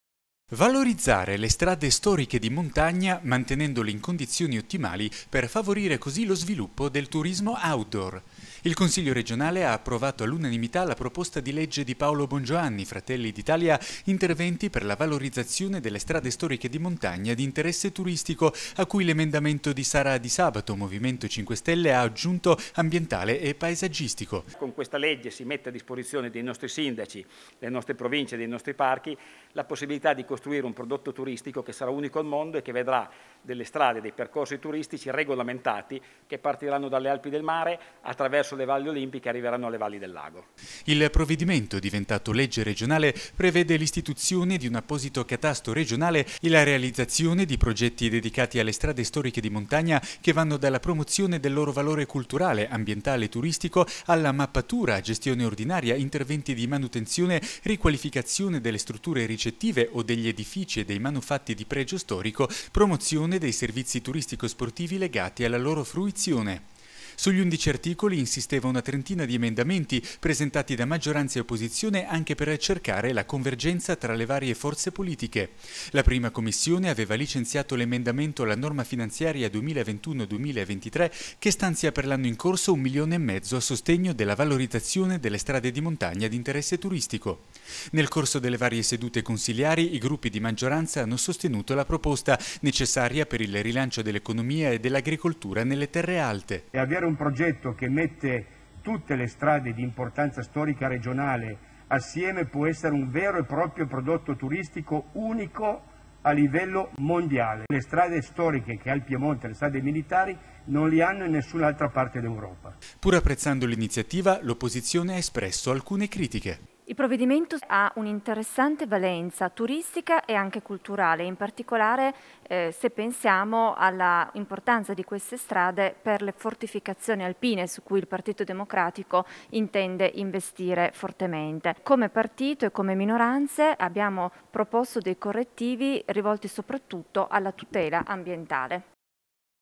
Strade storiche di montagna
Approvata all'unanimità la legge che promuove la valorizzazione delle strade storiche di montagna di interesse turistico con l’obiettivo di mantenere le stesse in condizioni ottimali, favorendo così lo sviluppo del turismo outdoor.